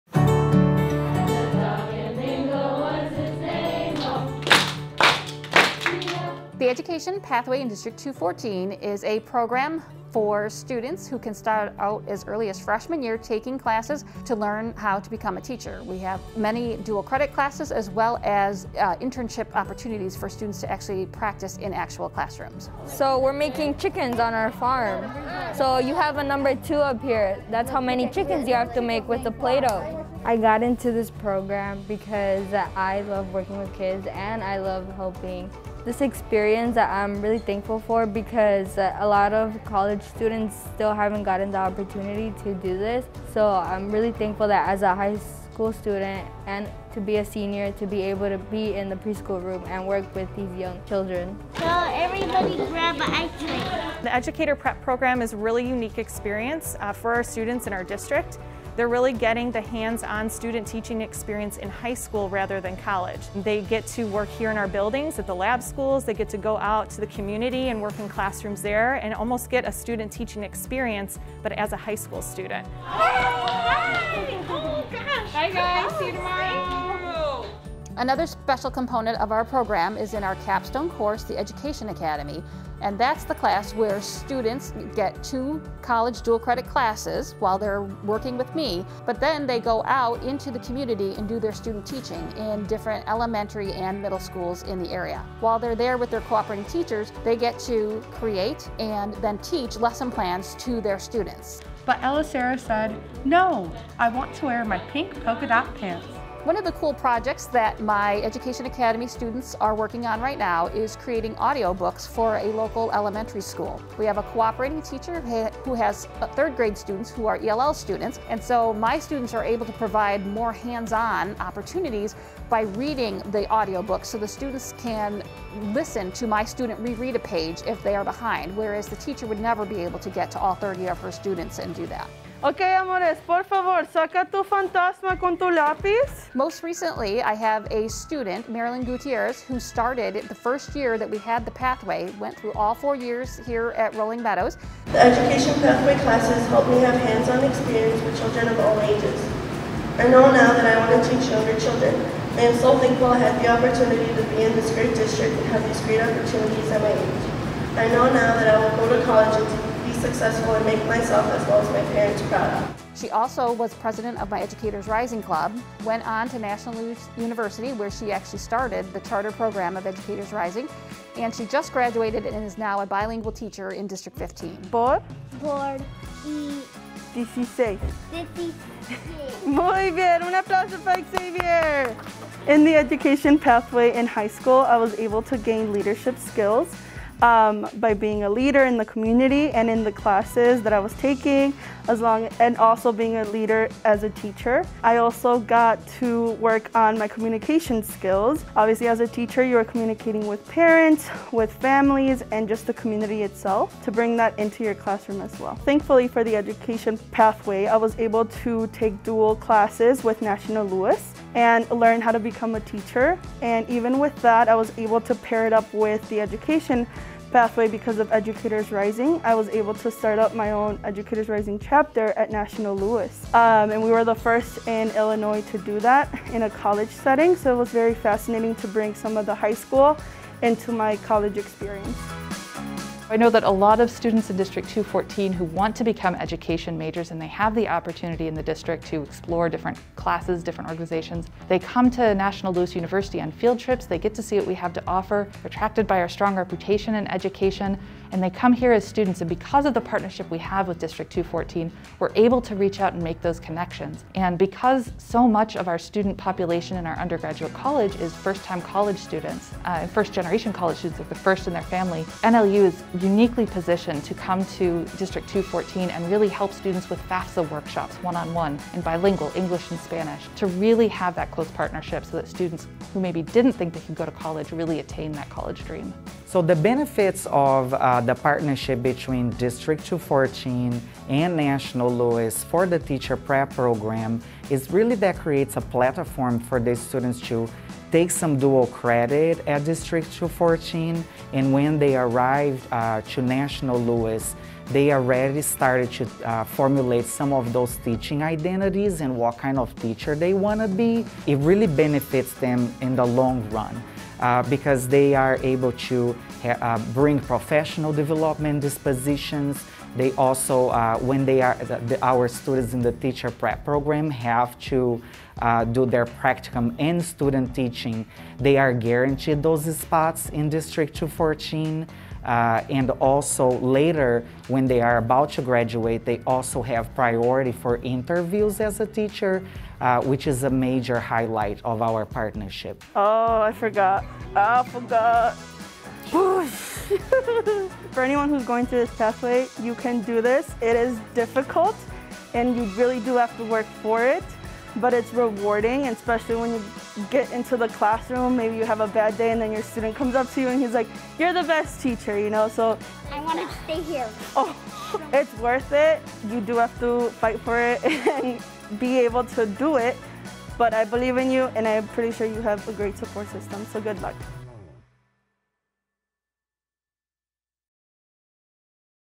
District 214’s Education Pathway Help Students Achieve Their Dreams
District 214’s Education Pathway Help Students Achieve Their Dreams.  High School District 214’s Career Pathways are designed to prepare students for success beyond high school by supporting their interest in a particular field or by pointing them in a different direction.